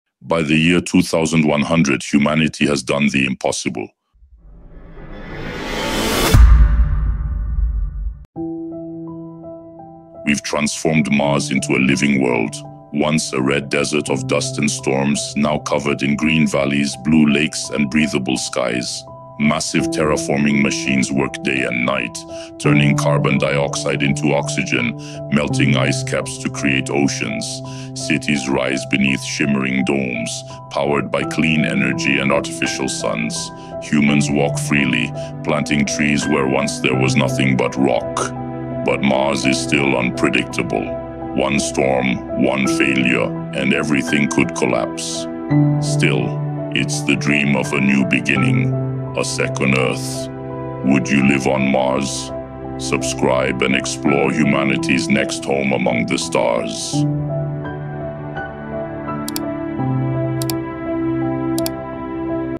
Terraforming Mars
Humans transforming Mars into a new Earth 🌍🪐 Oceans, forests, and cities on the Red Planet — the dream of terraforming Mars could soon become reality! 🚀